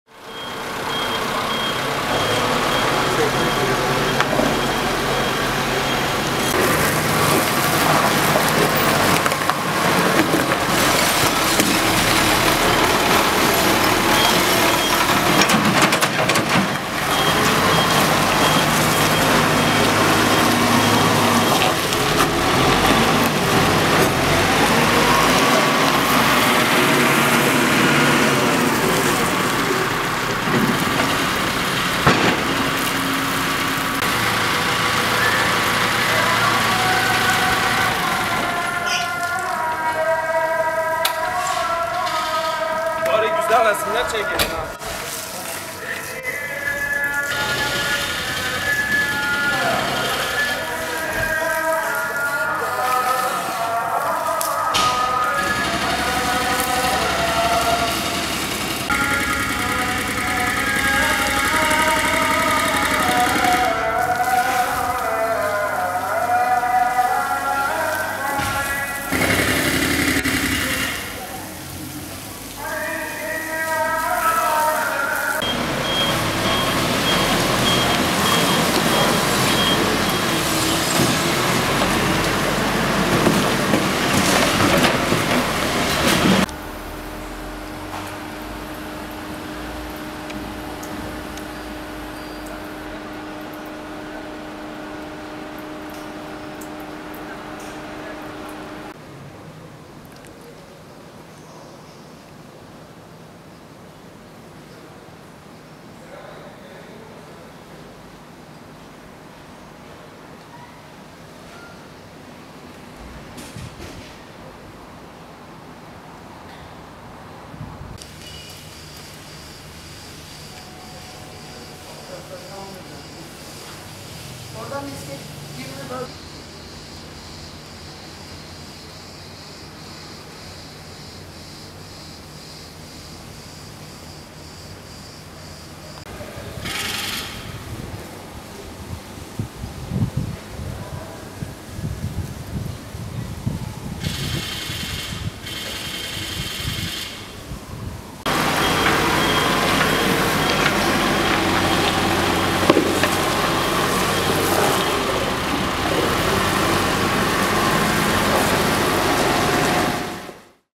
Aydın Efeler'deki Hacı Lütfiye Atay Ortaokulu'da Güçlendirme ve Yenileme Çalışmaları Yapılmaktadır.
Aydın Efeler'deki Hacı Lütfiye Atay Ortaokulu, öğrencilere güvenli ve modern bir ortam sunmak için 2029 Sokak'taki okulda bakım, onarım ve kolon güçlendirme çalışmaları yapılıyor. Proje kısa süre içinde tamamlanacak.